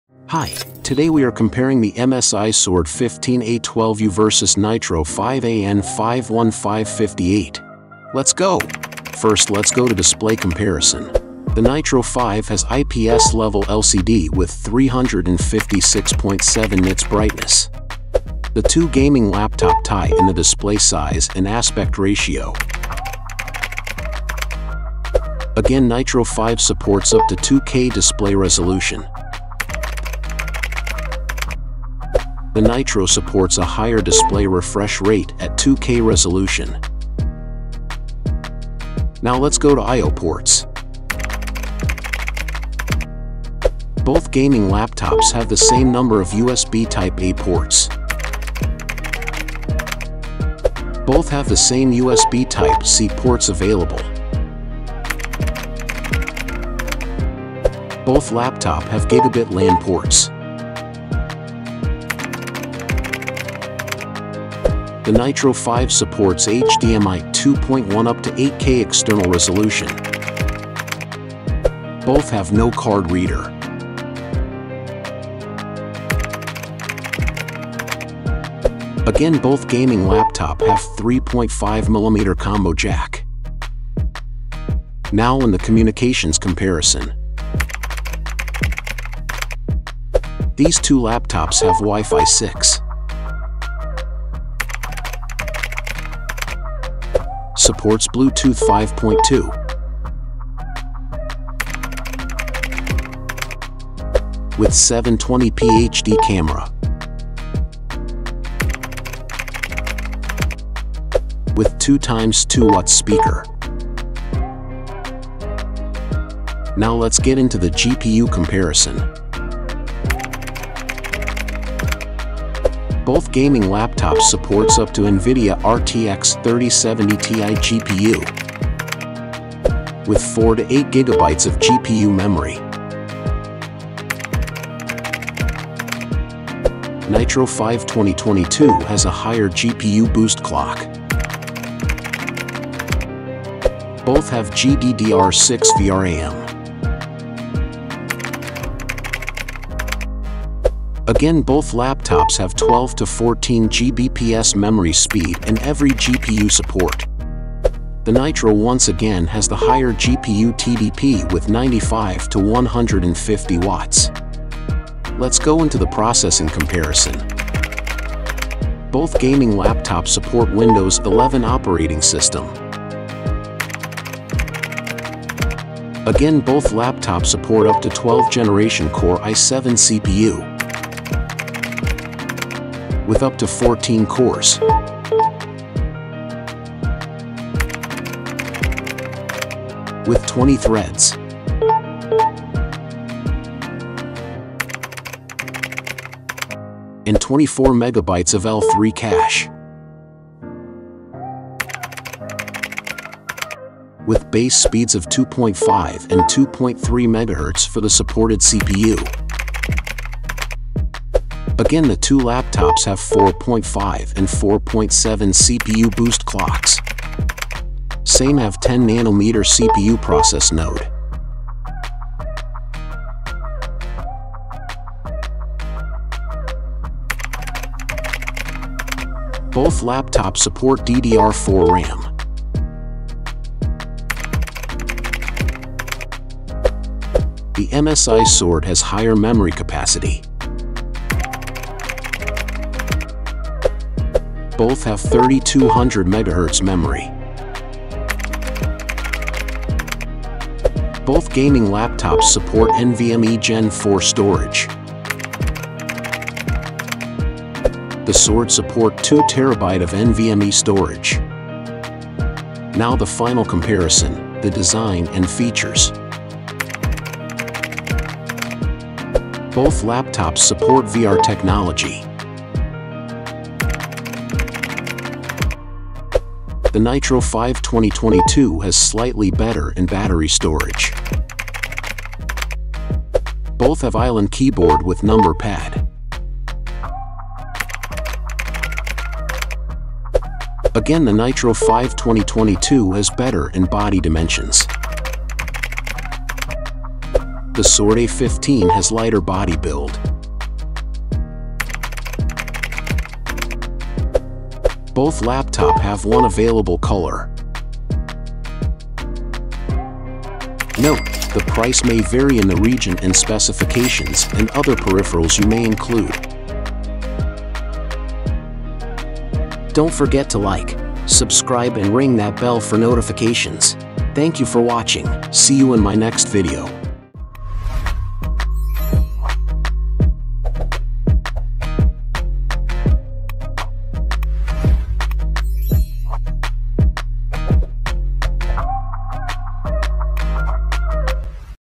Sword A15 VS Nitro 5 | 2022 | Tech Comparison P.7
💻 Technical and Specs Comparison between
MSI Sword 15 (2022) and Acer Nitro 5 AN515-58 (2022)
These are the best gaming laptop from the respected Manufacturer
All Laptops in this video are using Intel Mobile Processors
and NVIDIA Graphics Mobile Graphics Cards
Sword A15 vs Nitro 5 (2022) Editions

MSI vs Acer Product Comparison

Which should you buy, the Sword 15 or Acer Nitro 5 AN515-58? In this video, we compare and contrast these two gaming laptops so you can make the best decision for your needs.
Both of these laptops are great for gaming, but some key differences exist.

𝗡𝗼𝘁𝗲: 𝗔𝗹𝗹 𝗼𝗳 𝘁𝗵𝗲 𝗦𝗽𝗲𝗰𝘀 𝗼𝗳 𝗟𝗮𝗽𝘁𝗼𝗽 𝗼𝗻 𝘁𝗵𝗶𝘀 𝗩𝗶𝗱𝗲𝗼 𝗶𝘀 𝗕𝗮𝘀𝗲 𝗼𝗻 𝘁𝗵𝗲 𝗥𝗲𝘀𝗽𝗲𝗰𝘁𝗶𝘃𝗲 𝗠𝗮𝗻𝘂𝗳𝗮𝗰𝘁𝘂𝗿𝗲𝗿 𝗶𝗳 𝘆𝗼𝘂 𝗵𝗮𝘃𝗲 𝘀𝗼𝗺𝗲 𝗾𝘂𝗲𝘀𝘁𝗶𝗼𝗻 𝘆𝗼𝘂 𝗰𝗮𝗻 𝗴𝗼𝗼𝗴𝗹𝗲 𝘁𝗵𝗲 𝘀𝗽𝗲𝗰𝘀 𝗼𝗿 𝗴𝗼𝘁 𝘁𝗼 𝘁𝗵𝗲 𝗼𝗳𝗳𝗶𝗰𝗶𝗮𝗹 𝘄𝗲𝗯𝘀𝗶𝘁𝗲 𝗼𝗳 𝘁𝗵𝗲 𝗟𝗮𝗽𝘁𝗼𝗽 𝗠𝗮𝗻𝘂𝗳𝗮𝗰𝘁𝘂𝗿𝗲𝗿 𝘁𝗵𝗮𝗻𝗸 𝘆𝗼𝘂 𝗮𝗻𝗱 𝗞𝗲𝗲𝗽 𝗦𝗮𝗳𝗲.

⏰  Contents of this video:
Intro
Display
I/O Ports
GPU
Processing
Memory and Storage
Communications
Design and Features
Price Comparison

 CATEGORY DETAILS IN THIS VIDEO

🖥️ 𝗗𝗜𝗦𝗣𝗟𝗔𝗬 𝗖𝗔𝗧𝗘𝗚𝗢𝗥𝗬
SCREEN SIZE
SCREEN MAX RESOLUTION
SCREEN TYPE
REFRESH RATE
GRAPHICS CARD
GPU MEMORY SIZE
GPU BOOST CLOCK
GPU MEMORY SPEED
GPU MEMORY TYPE
GPU MEMORY BUS
GPU THERMAL DESIGN POWER
GPU PROCESS NODE

⚙️ 𝗜𝗢 𝗣𝗢𝗥𝗧𝗦 𝗖𝗔𝗧𝗘𝗚𝗢𝗥𝗬
VIDEO PORT
USB TYPE-A PORT
USB TYPE-C PORT
AUDIO PORT
LAN PORT
CARD READER

🔗 𝗣𝗥𝗢𝗖𝗘𝗦𝗦𝗜𝗡𝗚 𝗖𝗔𝗧𝗘𝗚𝗢𝗥𝗬
OPERATING SYSTEM (OS)
PROCESSOR
CPU BASE FREQUENCY
CPU TURBO FREQUENCY
CPU CORES
CPU THREADS
CPU L3 CACHE
CPU WATTAGE
CPU PROCESS NODE
MEMORY MAX CAPACITY
MEMORY TYPE
MEMORY CLOCK
MEMORY SLOT
STORAGE TYPE
TOTAL STORAGE SLOTS
STORAGE MAX CAPACITY

📡 𝗖𝗢𝗠𝗠𝗨𝗡𝗜𝗖𝗔𝗧𝗜𝗢𝗡𝗦 𝗖𝗔𝗧𝗘𝗚𝗢𝗥𝗬
WIFI
BLUETOOTH
SPEAKER
AUDIO
WEBCAM

🖼️ 𝗗𝗘𝗦𝗜𝗚𝗡 𝗔𝗡𝗗 𝗙𝗘𝗔𝗧𝗨𝗥𝗘𝗦 𝗖𝗔𝗧𝗘𝗚𝗢𝗥𝗬
VR READY
KEYBOARD TYPE
KEYBOARD NUMERIC
BATTERY
DIMENSIONS W-D-H
WEIGHT
COLOR

Laptop Price in Conversion with Other Country
US Dollar
Japan Yen
Australian Dollar
UK Pound
Germany Euro
Canadian Dollar

⚠️Warning Do not Click this Link: shorturl.at/chkDK